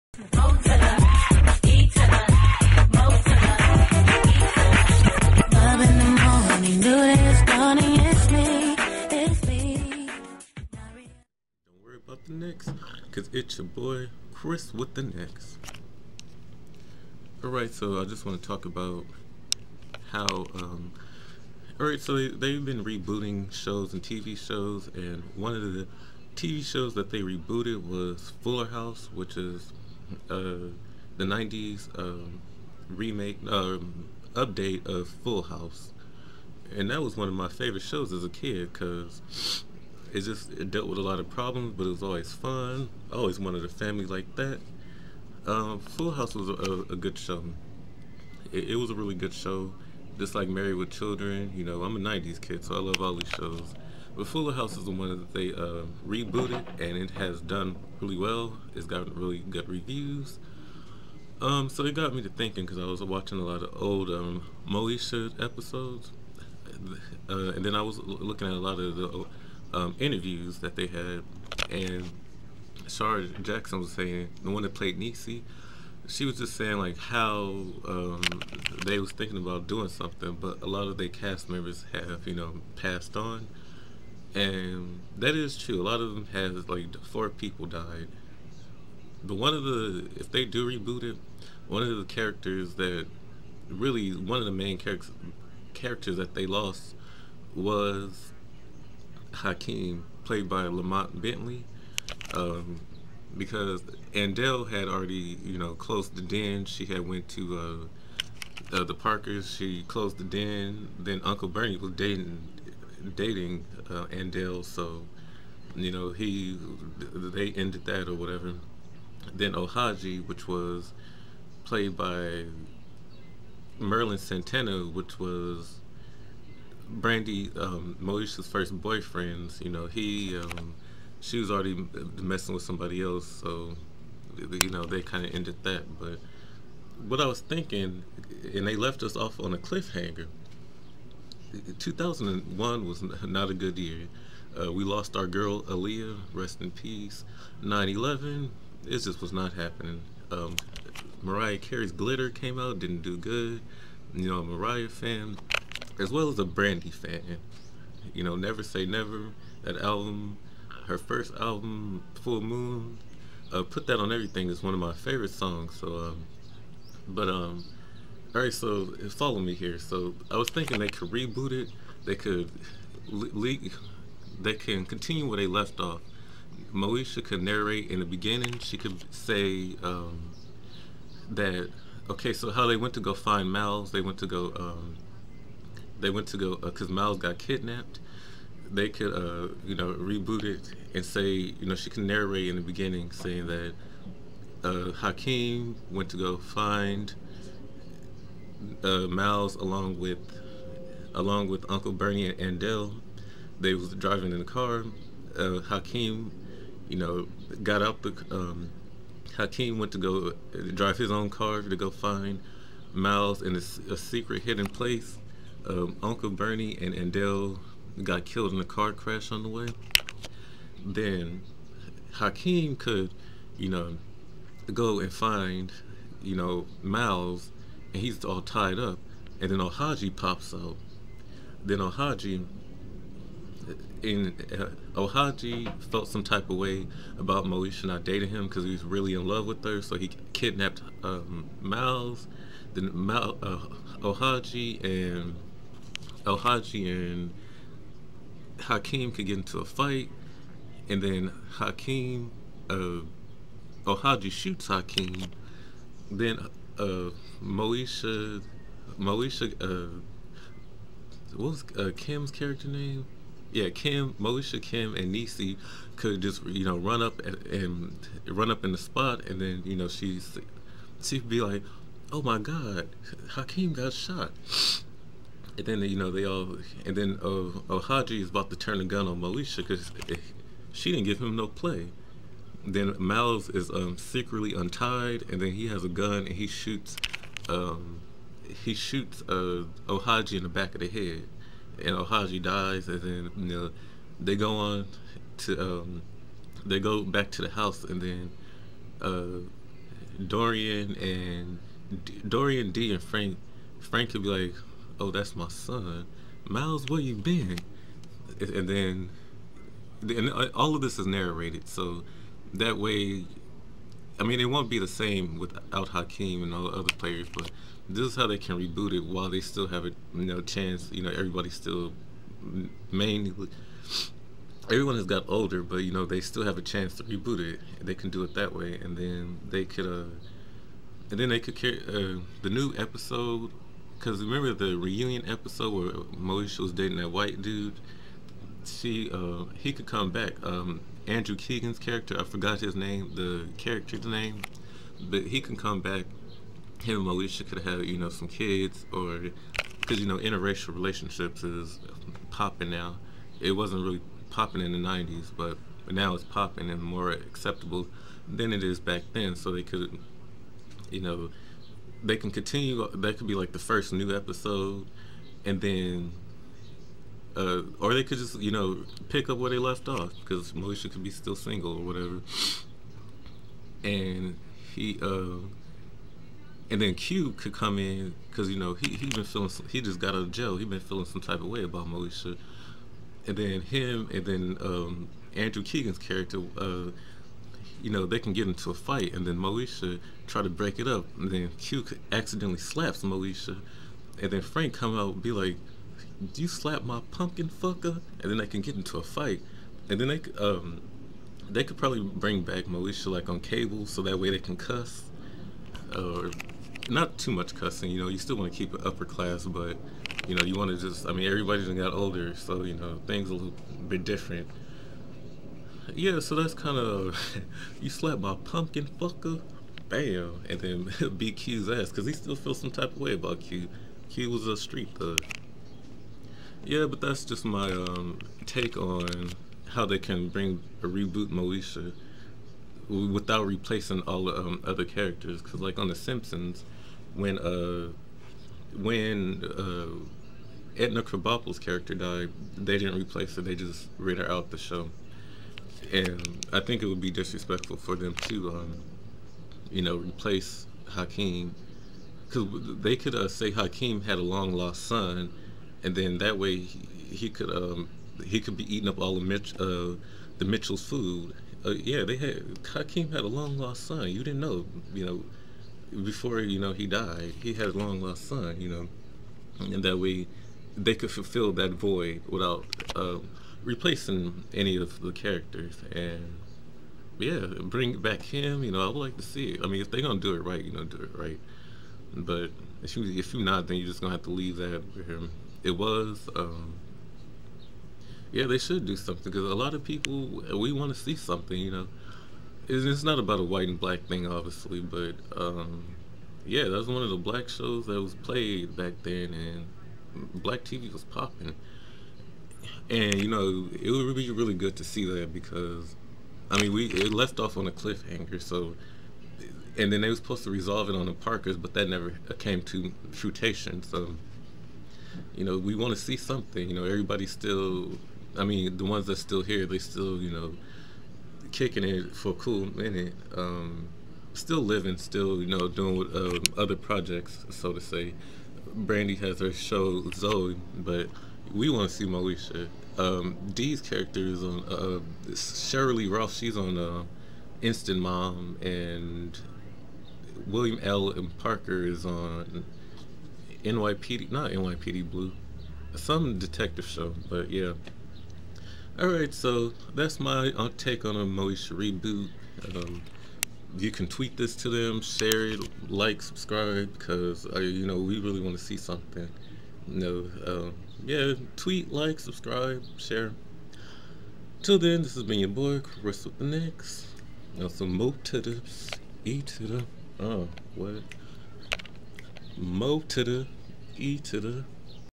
Moesha Reboot?
This is my take on how they can bring back Moesha! These are just some ideas that i have, I know its grim but think about it or if you got a better idea leave it in the comment section below!